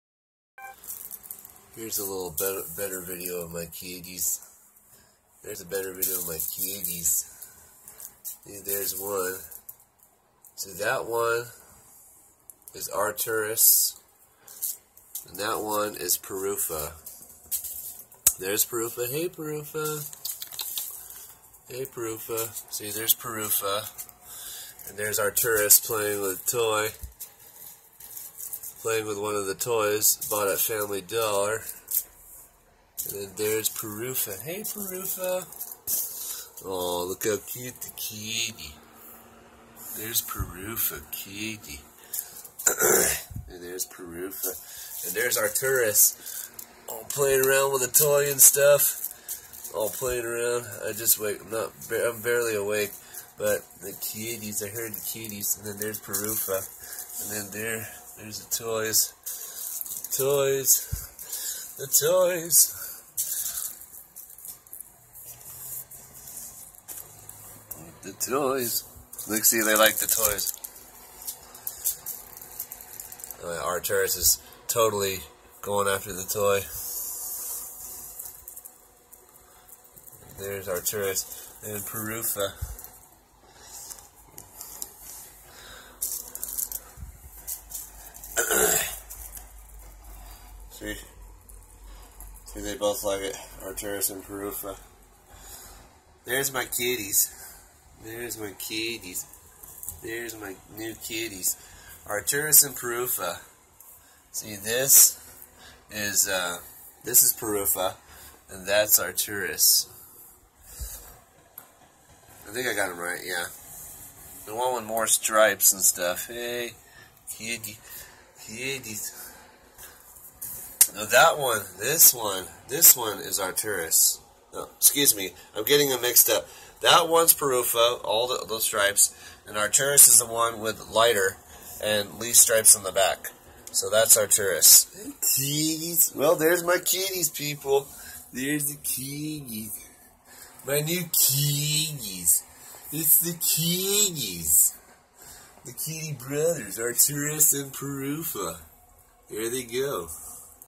My kitties on the morning
Dane the destroyer me recording g my new kitties In the morning. Arturis and Purufa.